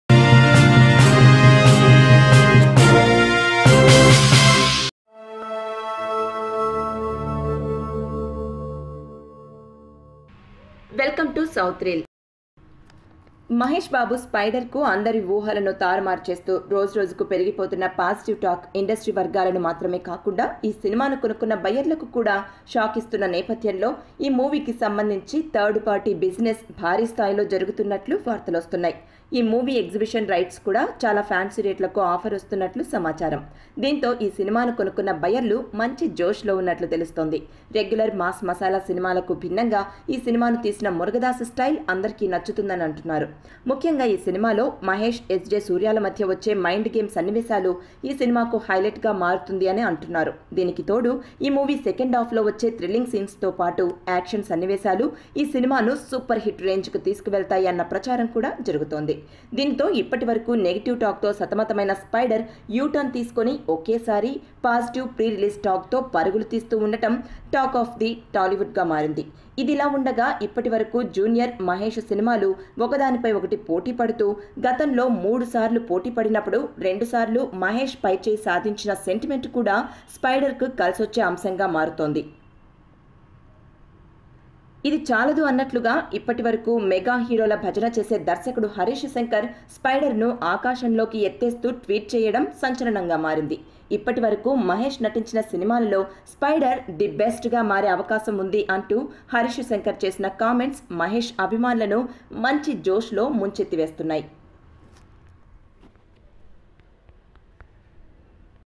స్పైడర్ మూవీ ని ఆకాశానికి ఎత్తేసిన హరీష్ శంకర్ | Harish Shankar Shocking Comments On Spyder Movie
Watch     స్పైడర్ మూవీ ని ఆకాశానికి ఎత్తేసిన హరీష్ శంకర్ | Harish Shankar Shocking Comments On Spyder Movie